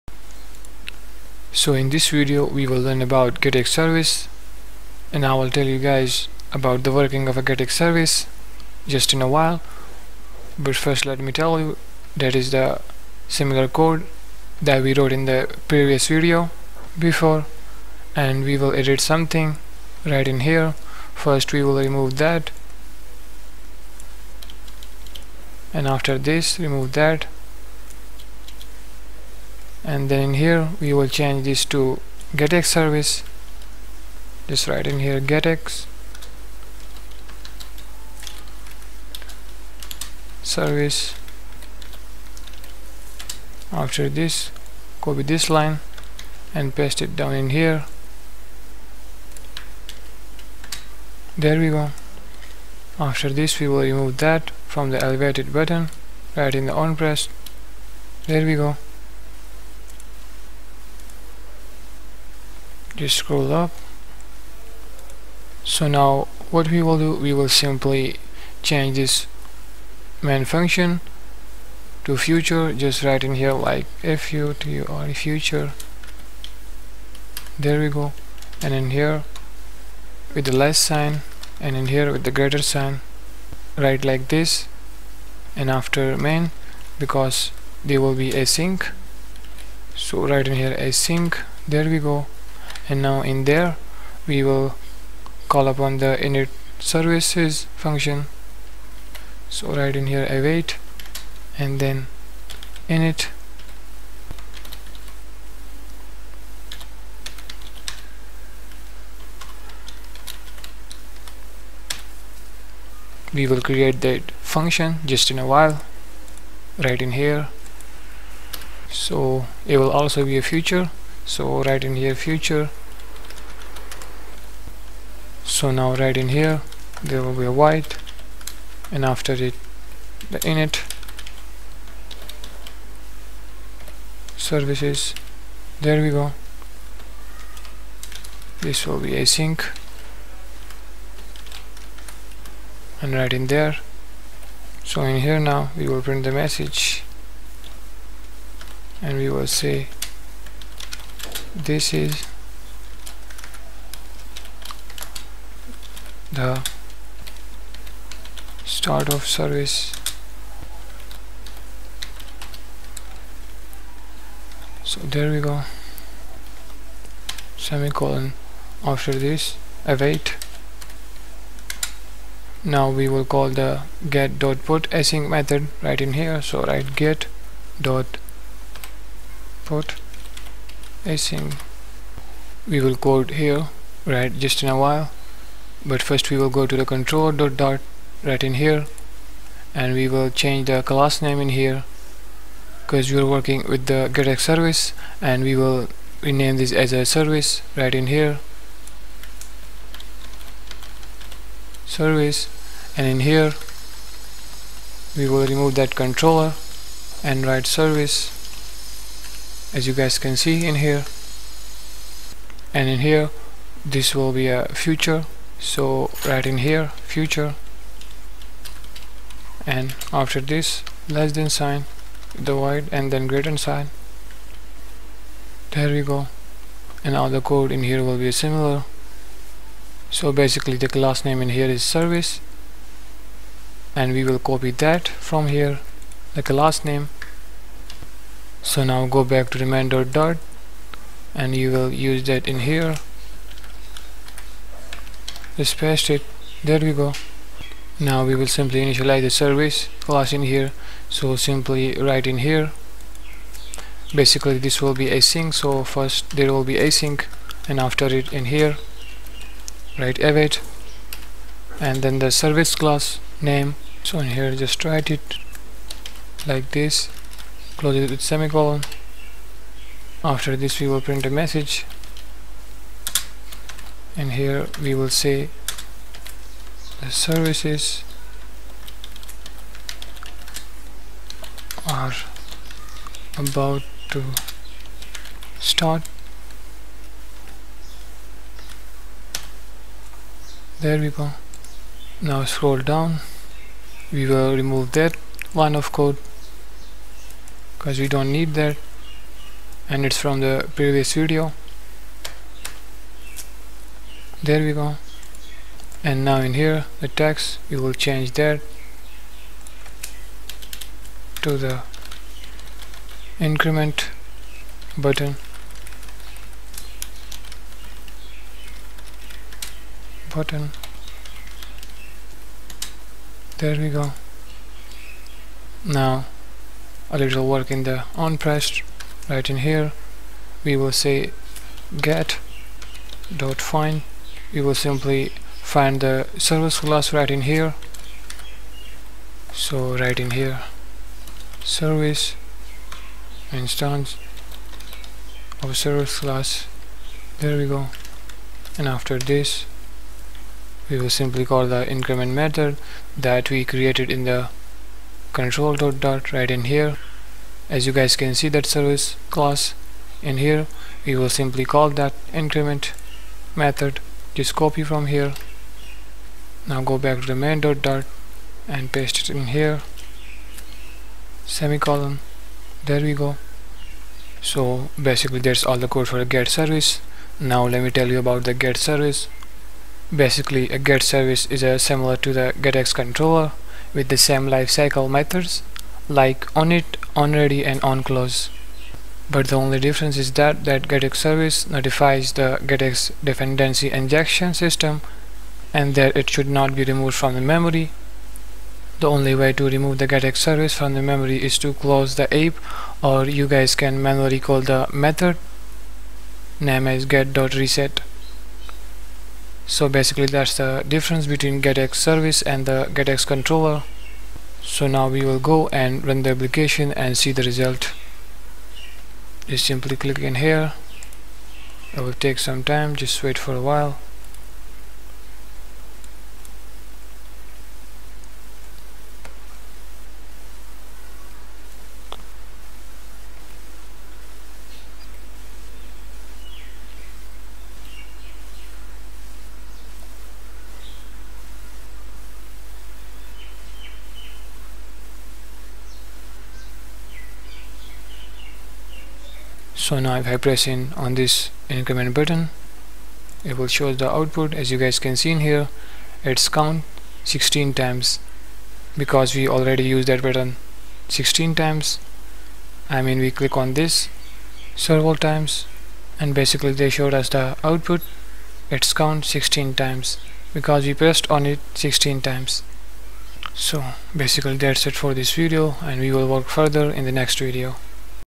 GetX Services Tutorial 2022 - GetX Service Flutter 2.5 iOS and Android App Development Full Course
This is the video#12 number first of Flutter GetX Full Course. In this video you will learn what is getx service in flutter.